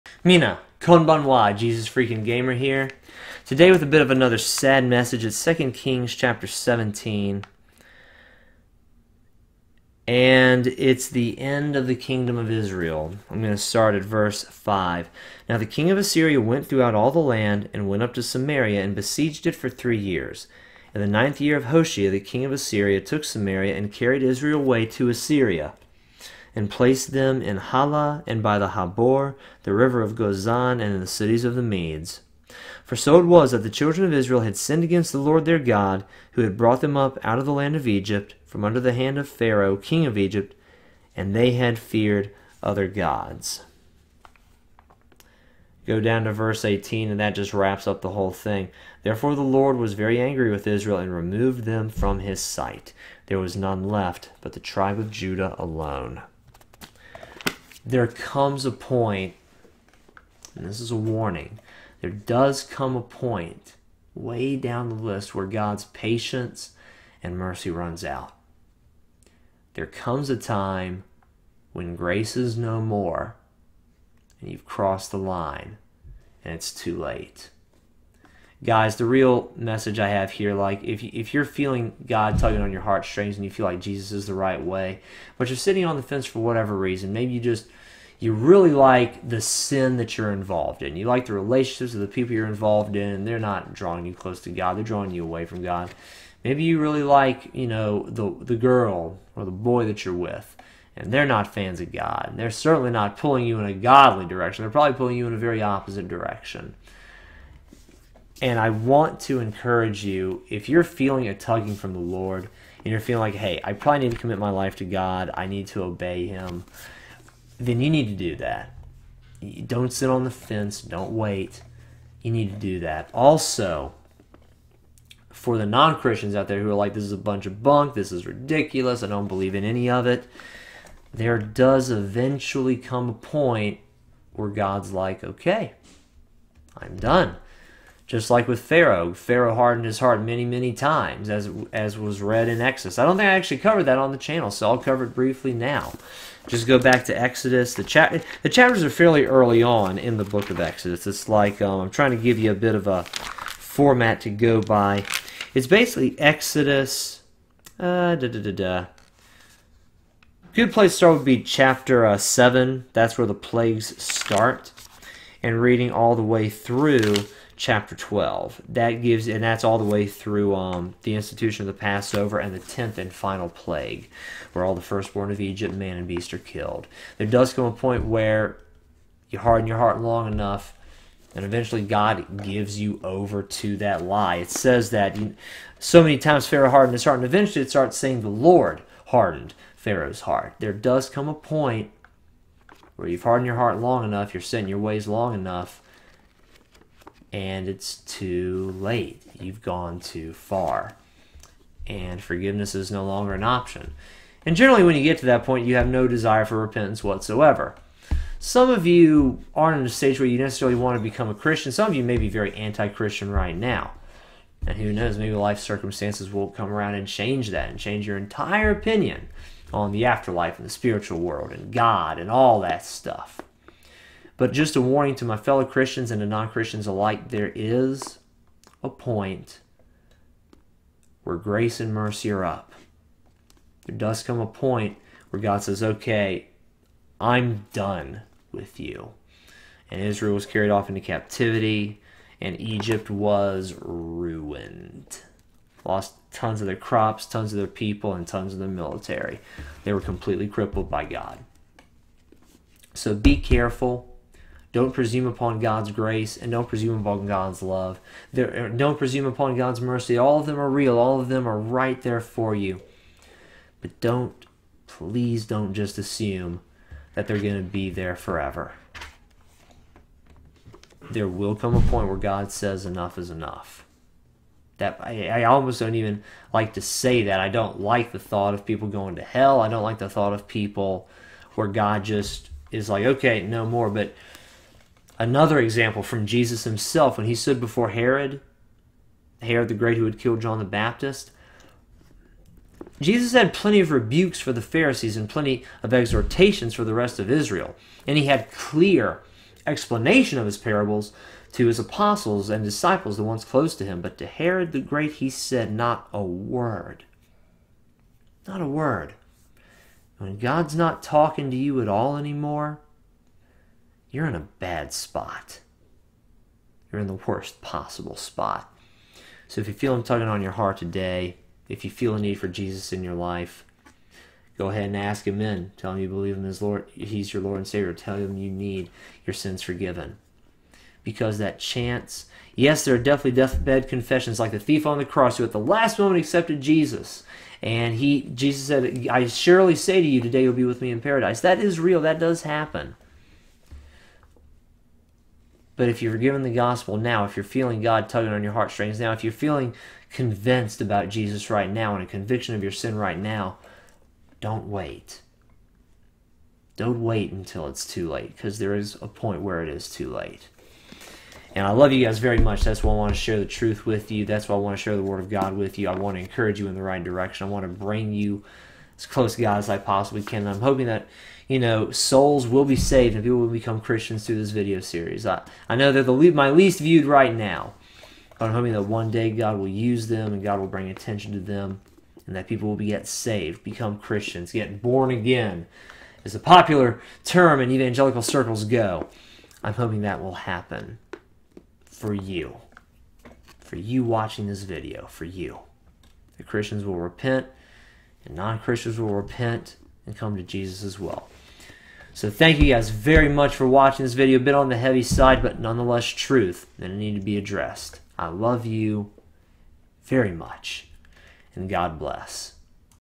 THERE IS A POINT OF NO RETURN
There comes a time when it's too late. Please don't wait until that time, because then you'll want to change and you won't be able to. There even comes a point where God won't listen